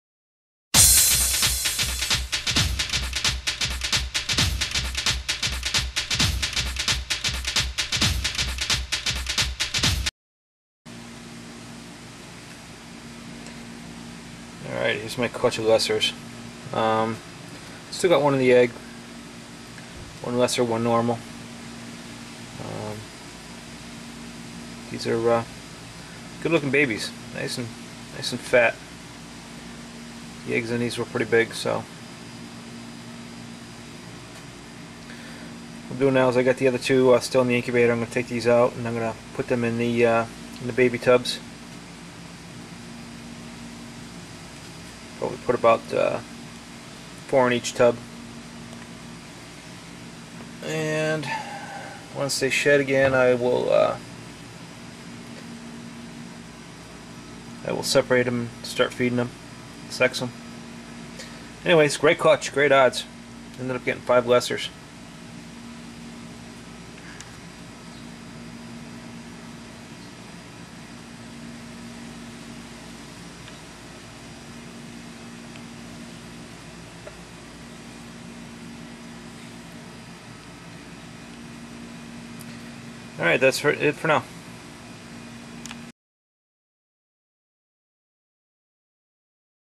Lesser Clutch
Ball Python Lesser clutch out of the egg and ready for the baby box.